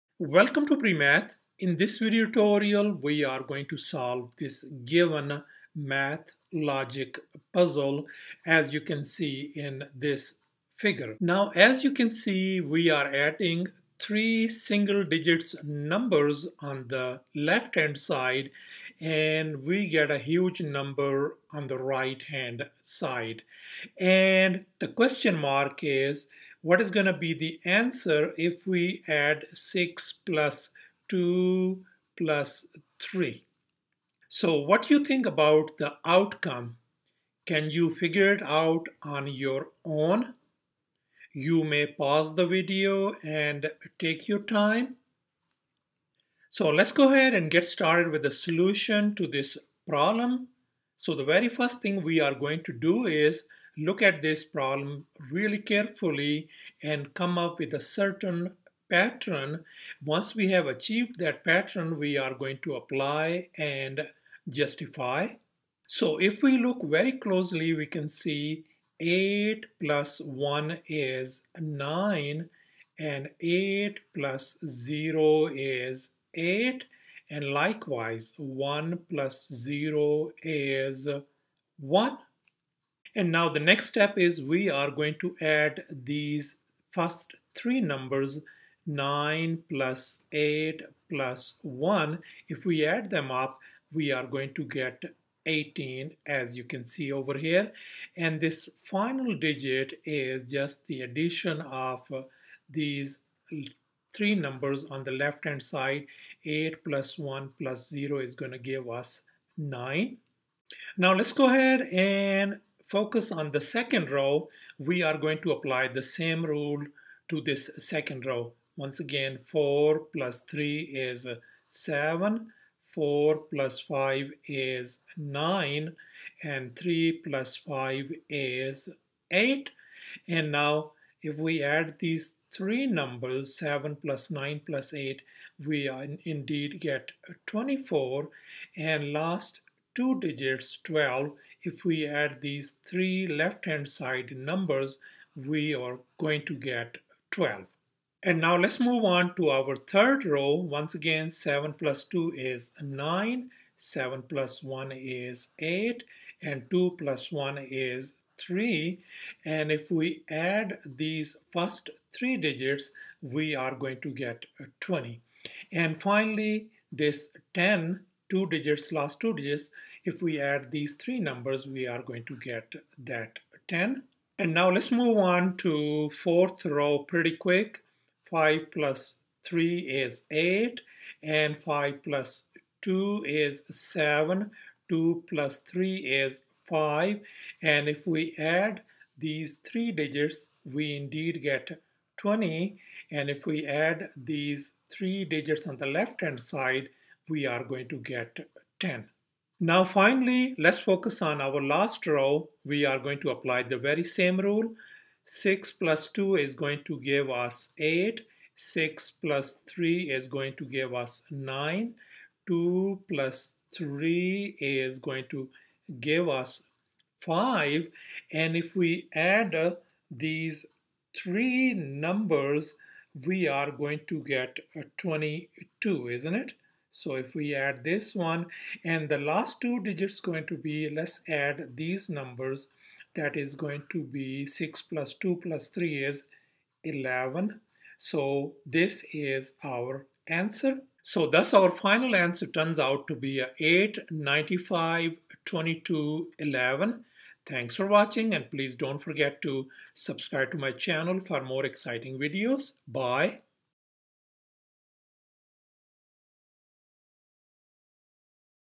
How Fast Can You Solve This Logic Puzzle?
Learn how to solve this logic math puzzle. Step-by-step explanation by PreMath.com